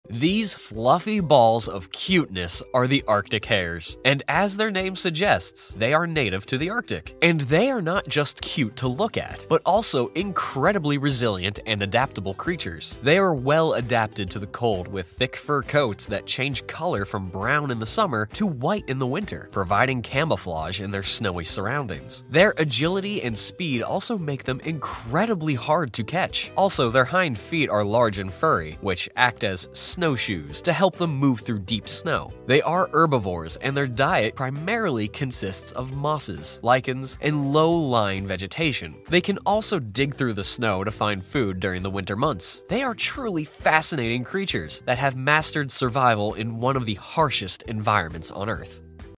Arctic Hare 🐰 The Cutest Snowball Of The Arctic!
The Arctic hare is a species of hare that is well adapted to living in the harsh, cold climates of the Arctic tundra. They have thick, white fur that helps them blend in with the snow and stay warm in freezing temperatures. Their large, furry feet also act as snowshoes, allowing them to move easily over deep snow. Arctic hares are herbivores, feeding on grasses, mosses, and other plant material, and they are an important prey species for many predators in the Arctic ecosystem.